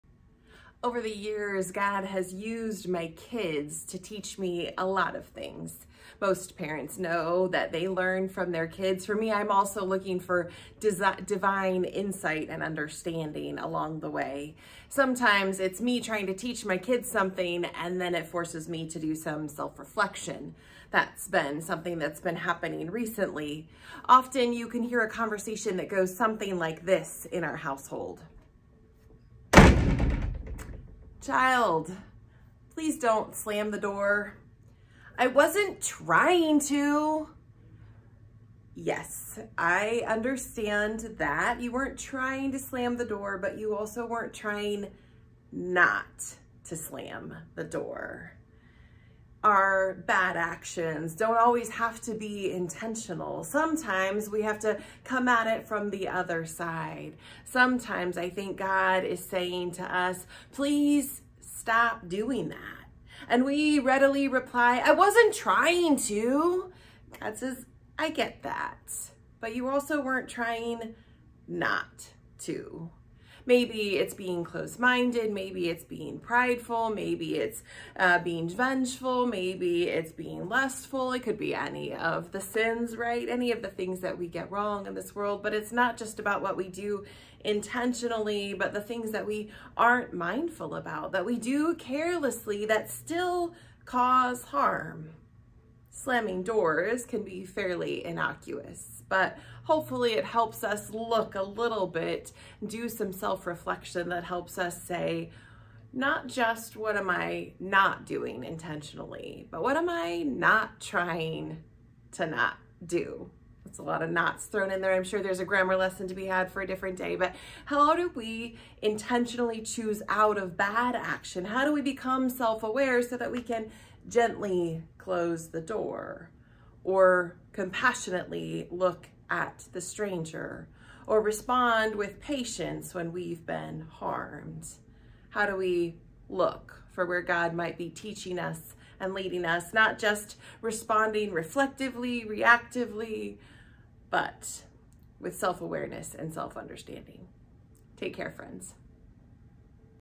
Intent
Debbie shares a lesson from a simple interaction with her kids. The negative things we do are not always intentional. How can we add intent to our actions so that we are reacting with self-awareness and compassion? How can we increase our mindfulness so that we can avoid mindless malicious actions?